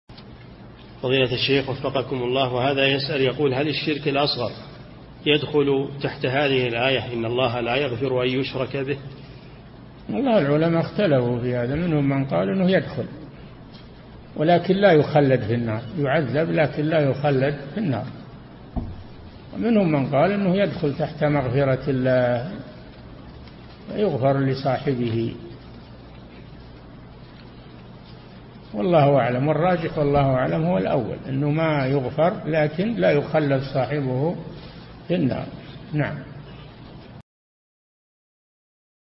[270 -939] هل الشرك الأصغر يدخل في قوله تعالى (إن الله لا يغفر أن يشرك به)؟ - الشيخ صالح الفوزان
-[270 -939] السؤال: هذا يسأل هل الشرك الأصغر يدخل تحت هذه الآية: "إن الله لا يغفر أن يشرك به"؟
الجواب: العلماء اختلفوا في هذا، فمنهم من قال إنه يدخل ولكن لا يخلد في النار يعذب ولكن لا يخلد في النار، ومنهم من قال إنه يدخل تحت مغفرة الله ويغفر لصاحبه، والله أعلم! والراجح -والله أعلم!- هو الأول أنه ما يغفر لكن لا يخلد صاحبه في النار.
للاشتراك في قناة الشيخ صالح الفوزان

للاستفسار والإقتراحات والدعم
رئيس الفريق
لمشروع كبار العلماء